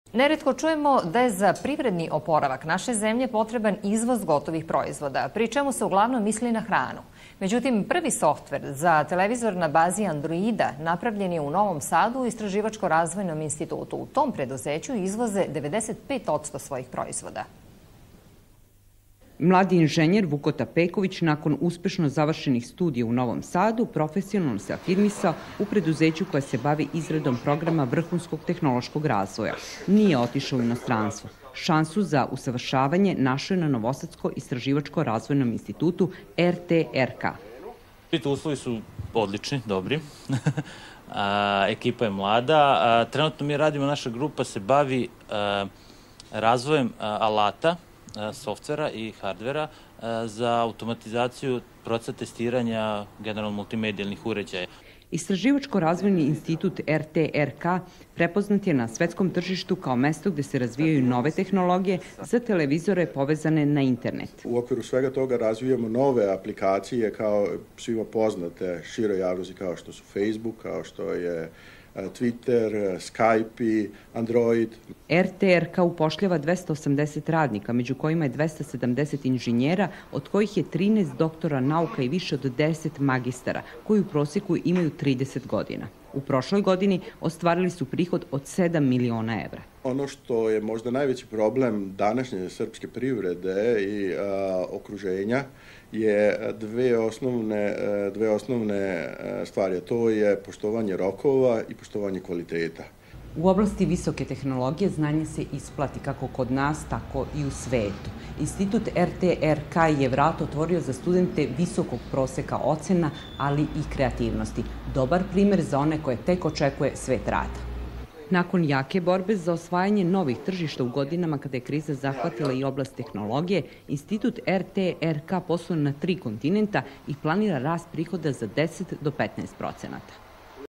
RT RK in Public News
RT-RK is recently accredited as a national research and development institute by the Serbian Ministry of Science and Technological Development.

More than 270 young engineers are a good example of how the dedicated work and use of modern equipment can thrive professionally even in the years of great economic and economic crisis.

The video was recorded by a team of local Radio Television of Vojvodina and then aired on national television.